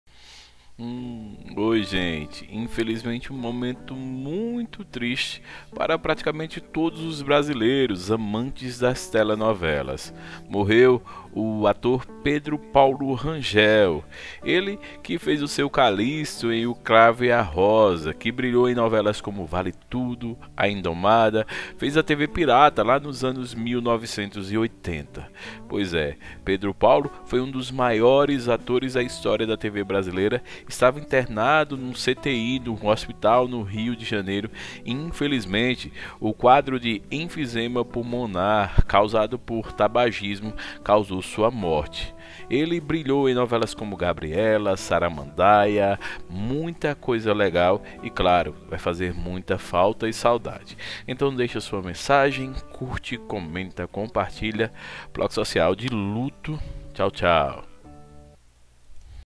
LUTO na GLOBO • Morre o ator Pedro Paulo Rangel, o Seu Calixto de O cravo e a rosa
Morreu no fim da madrugada desta quarta-feira (21) o ator Pedro Paulo Rangel. A informação foi confirmada pela família.

O artista de 74 anos estava internado no CTI da Casa de Saúde São José, na Zona Sul do Rio, para tratar uma descompensação do quadro de enfisema pulmonar, desde o dia 30 de novembro. A causa da morte ainda não foi divulgada.

Entre os trabalhos mais marcantes de Pedro Paulo estão as novelas "Gabriela" (1975), "Saramandaia" (1976), "Vale Tudo" (1988), e o humorístico "TV Pirata" (1988).

SIGA O APRESENTADOR: 👉 INSTAGRAM: @RayhBloom